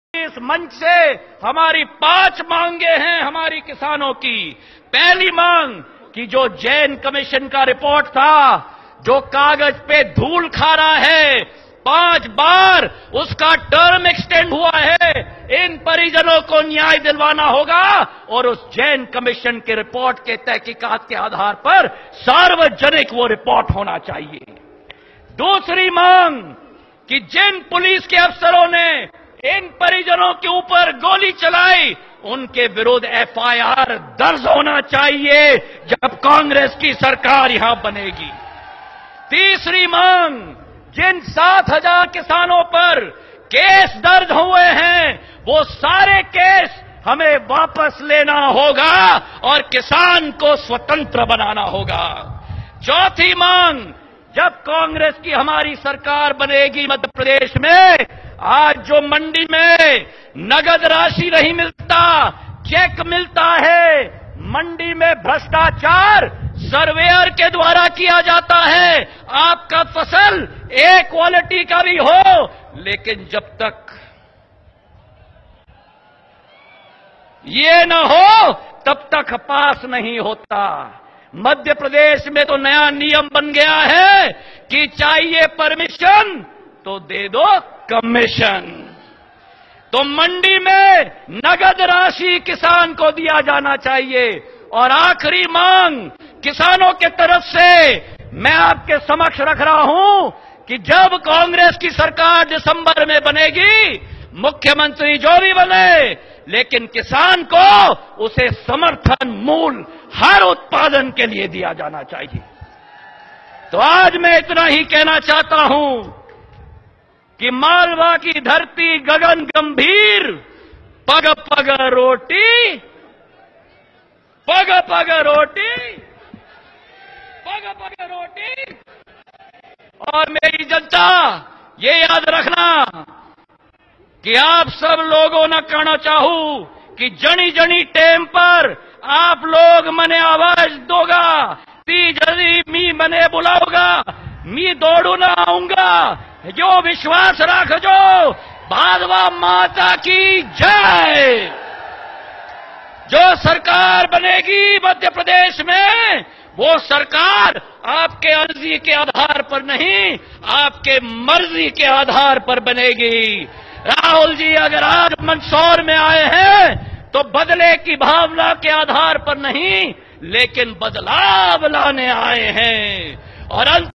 Jyotiraditya M Scin....
हमारे किसानों के लिए हमारी 5 मांगे है - जैन कमीशन की रिपोर्ट सार्वजानिक की जाये, दोषी पुलिसकर्मी के खिलाफ FIR दर्ज हों, 7000 किसानों के खिलाफ दर्ज मुक़दमे वापस लिए जायें, मंडी में नकद राशि में भुगतान किया जाए, और हर उत्पादन के लिए समर्थन मूल्य दिया जाए